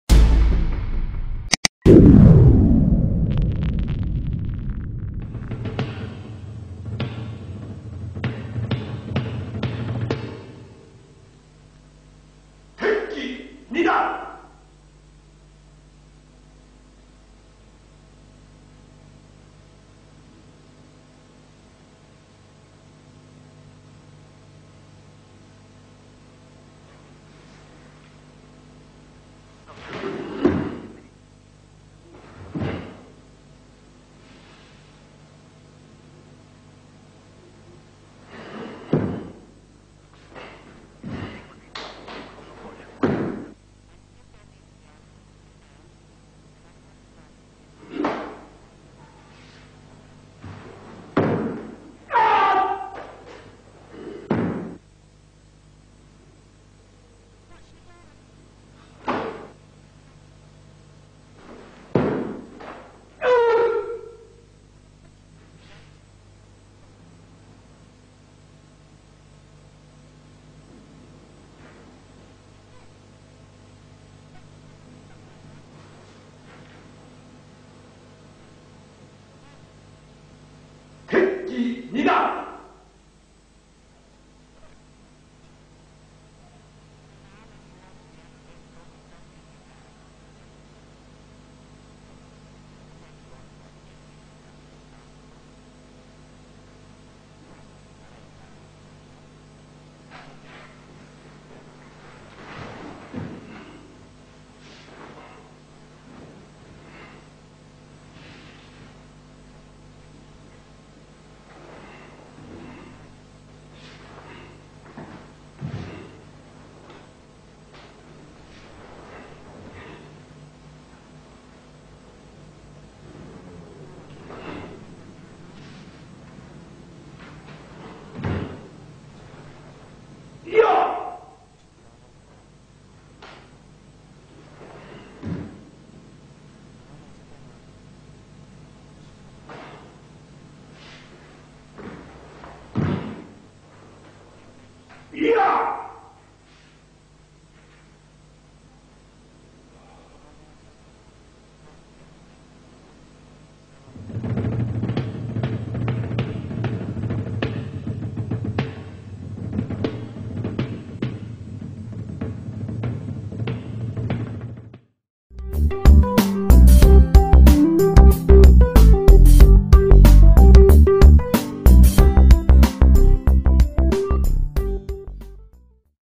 KATA TEKKI NIDAN By HIROKAZU KANAZAWA 4K 60FPS
TEKKI NIDAN :

Tekki Nidan  is the shortest Tekki kata, consisting of  24 movements. Similar to Tekki Shodan in many ways, this kata is also performed almost entirely in horse stance, with the exception of a few crossing steps and one heisoku-dachi. The objective in this kata is to develop grabbing, throwing, and locking applications while maintaining a strong kiba-dachi. To accomplish this, the student is introduced to many new techniques such as ryo-zenwan-suhei-kamae, zenwan-shomen-gedan-kensei, and oshi-uke. The use of tsukami-uke in this kata teaches how to draw an opponent in for counter-attack.